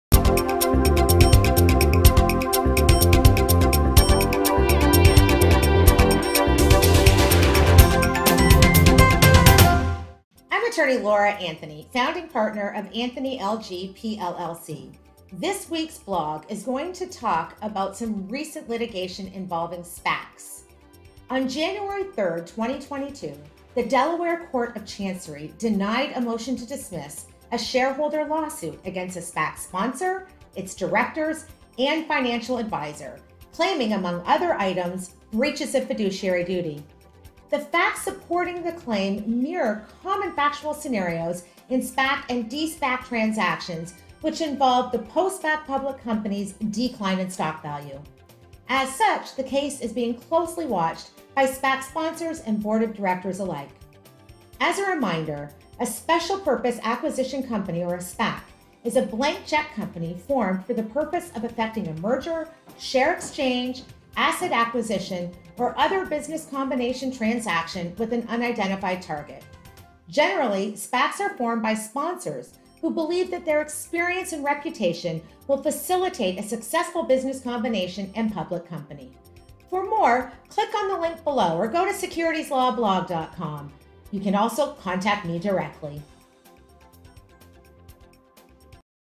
SPAC Shareholder Litigation - First Fire
SPAC Shareholder Litigation - First Fire- On January 3, 2022, the Delaware Court of Chancery denied a motion to dismiss a shareholder lawsuit against a SPAC’s sponsor, its directors, and financial advisor claiming among items, breach of fiduciary duty.  The facts supporting the claim mirror common factual scenarios in SPAC and de-SPAC (acquisition transaction) transactions where the post SPAC public company has a decline in stock value..